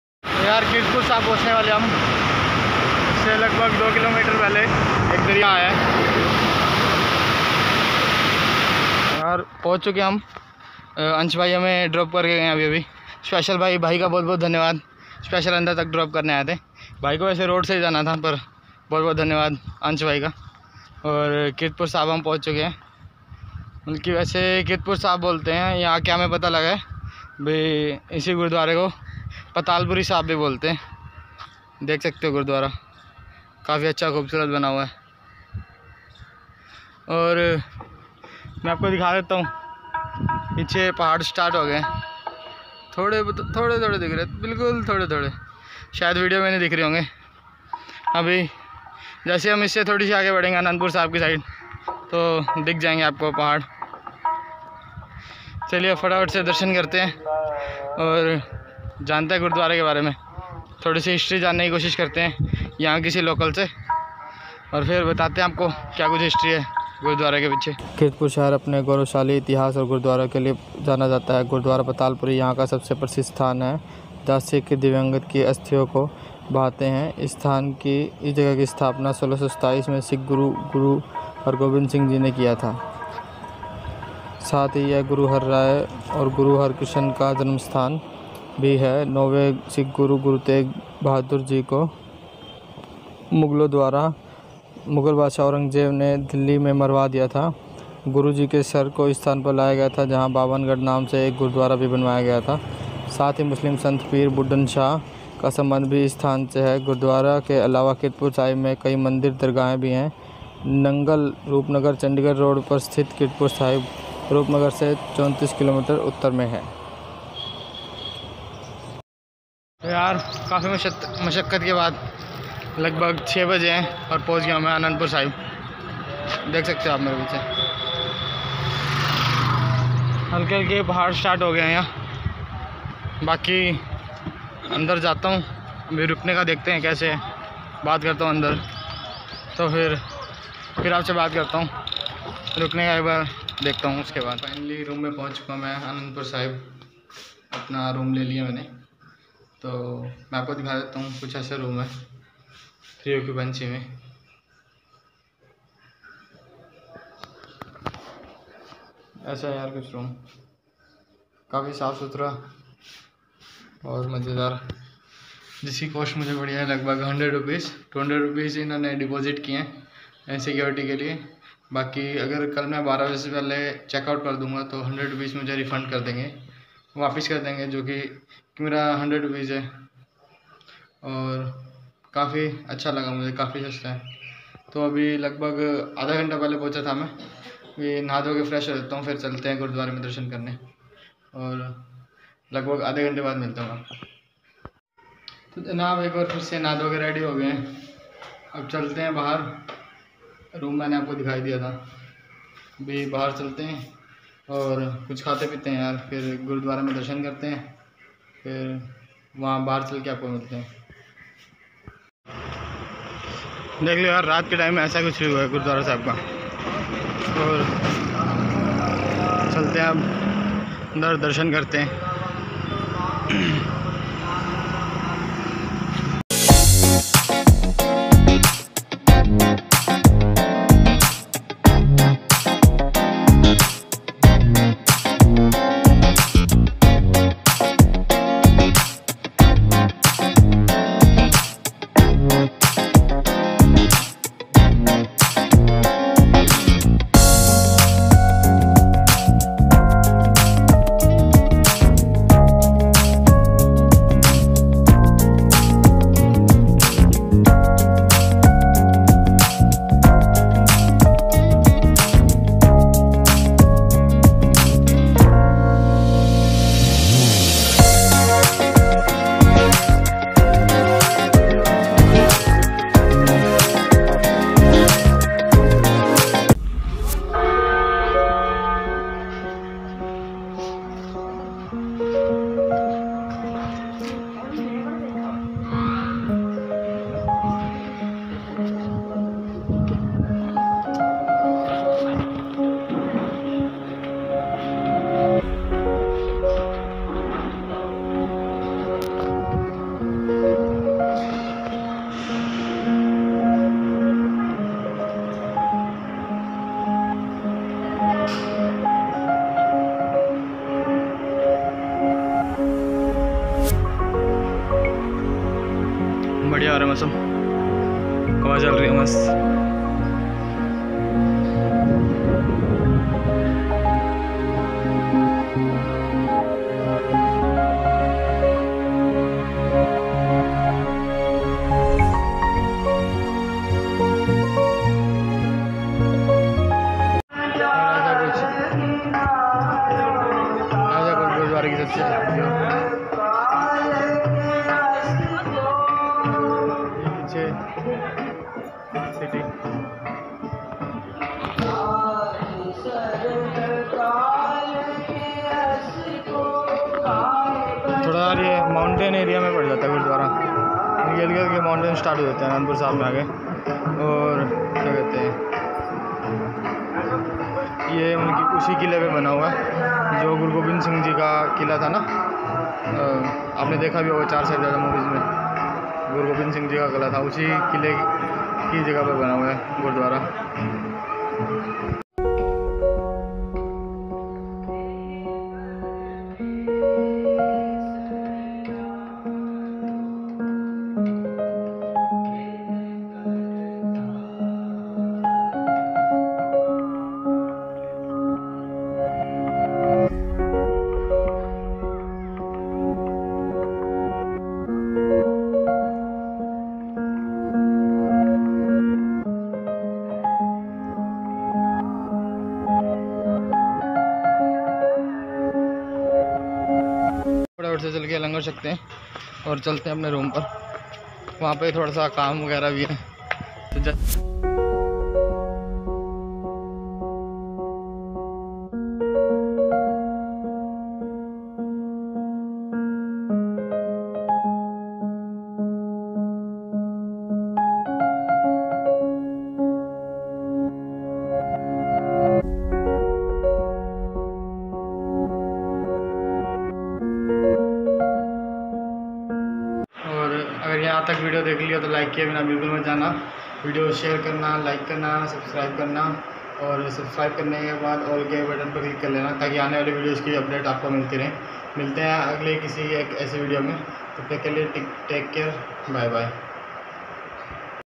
Kirtpur Sahib || Anandpur Sahib || Part 2 || Tourestic Indian || Hichhiking to Anandpur Sahib
In this video we can travel to  Kirtpur Sahib and Anandpur Sahib.

 Tourestic Indian
Facebook page Tourestic Indian